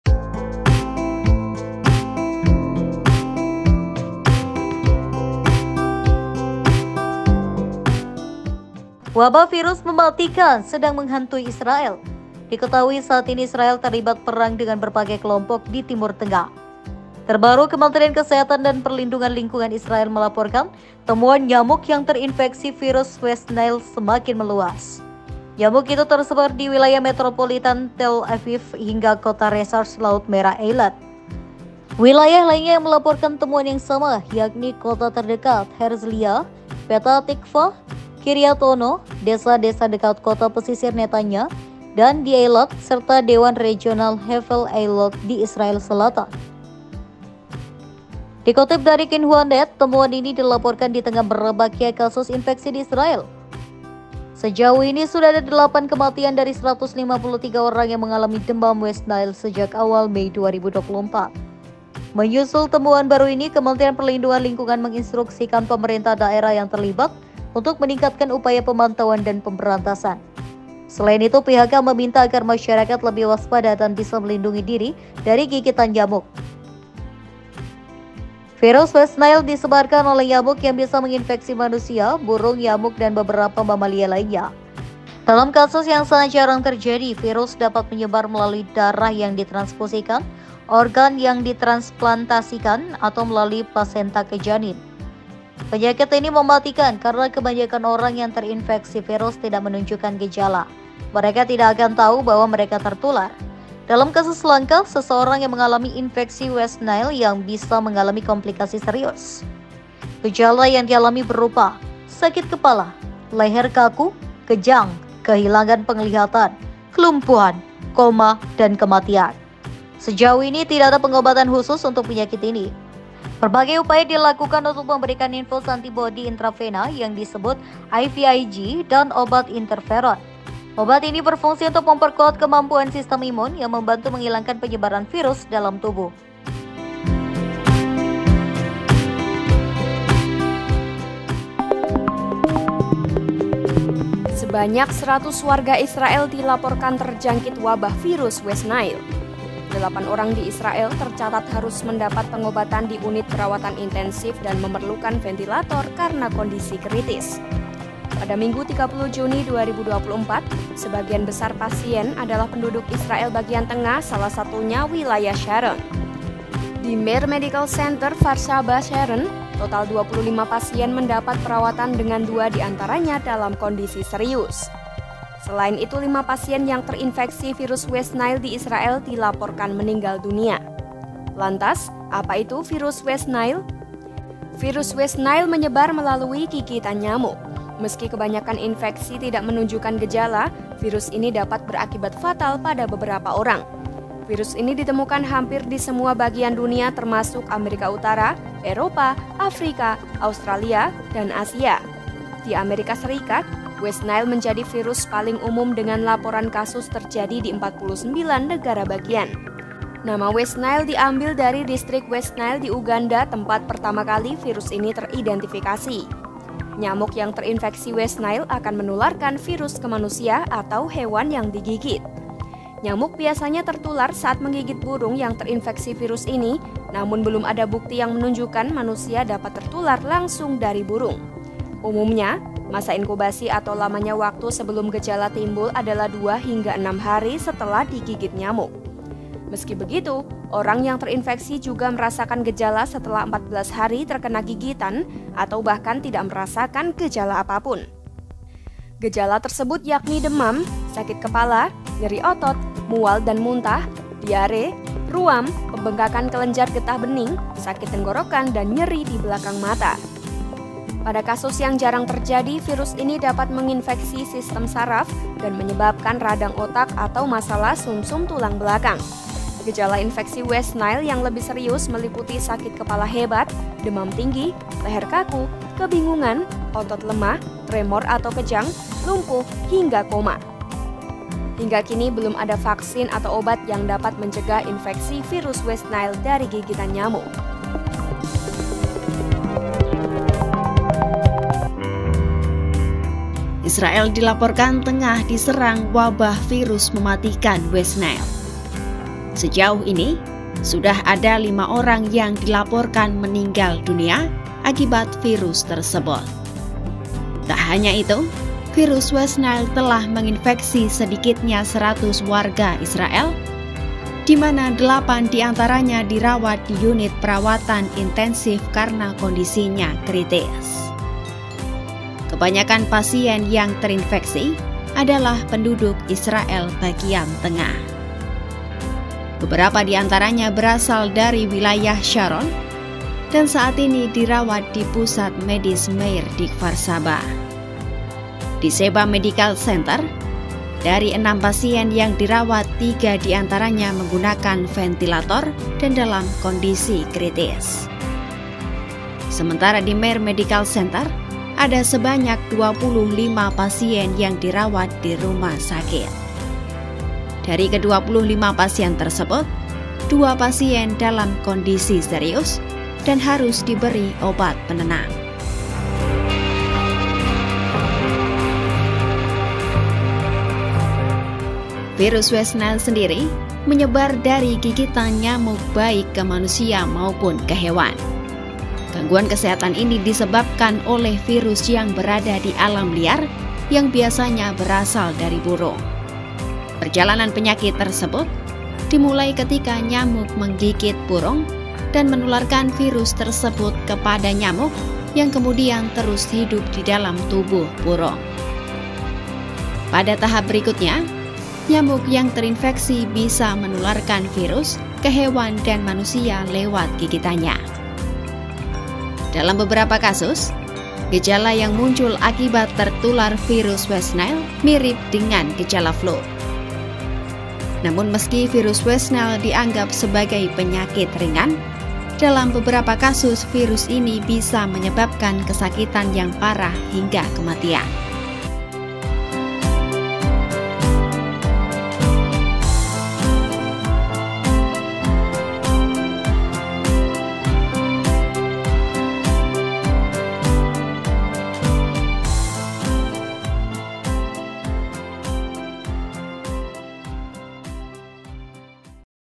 Lagi Perang, Israel Dilanda Wabah Penyakit Mematikan, Belum Ada Obatnya
Wabah virus mematikan sedang menghantui Israel.

Diketahui, saat ini Israel terlibat perang dengan berbagai kelompok di Timur Tengah.

Terbaru, Kementerian Kesehatan dan perlindungan lingkungan Israel melaporkan temuan nyamuk yang terinfeksi virus West Nile semakin meluas.

Nyamuk itu tersebar di wilayah metropolitan Tel Aviv hingga kota resor Laut Merah Eilat.

Wilayah lainnya yang melaporkan temuan yang sama, yakni kota terdekat Herzliya, Petah Tikva, Kiryat Ono, desa-desa dekat kota pesisir Netanya, dan di Eilat serta Dewan Regional Hevel Eilot di Israel selatan.

Dikutip dari Xinhua Net, temuan ini dilaporkan di tengah merebaknya kasus infeksi di Israel.

Sejauh ini, sudah ada delapan kematian dari 153 orang yang mengalami demam West Nile sejak awal Mei 2024.

Menyusul temuan baru ini, Kementerian Perlindungan Lingkungan menginstruksikan pemerintah daerah yang terlibat untuk meningkatkan upaya pemantauan dan pemberantasan.

Selain itu, pihaknya meminta agar masyarakat lebih waspada dan bisa melindungi diri dari gigitan nyamuk.

Virus West Nile disebarkan oleh nyamuk yang bisa menginfeksi manusia, burung, nyamuk, dan beberapa mamalia lainnya.

Dalam kasus yang sangat jarang terjadi, virus dapat menyebar melalui darah yang ditransfusikan, organ yang ditransplantasikan, atau melalui plasenta ke janin.

Penyakit ini mematikan karena kebanyakan orang yang terinfeksi virus tidak menunjukkan gejala.

Mereka tidak akan tahu bahwa mereka tertular.

Dalam kasus langka, seseorang yang mengalami infeksi West Nile bisa mengalami komplikasi serius.

Gejala yang dialami berupa:

Sakit kepala

Leher kaku

Kejang

Kehilangan penglihatan

Kelumpuhan

Koma

Kematian

Sejauh ini, tidak ada pengobatan khusus untuk penyakit ini.

Berbagai upaya dilakukan untuk memberikan infus antibodi intravena yang disebut IVIG dan obat interferon.

Obat ini berfungsi untuk memperkuat kemampuan sistem imun, yang membantu menghilangkan

Check It Babe..!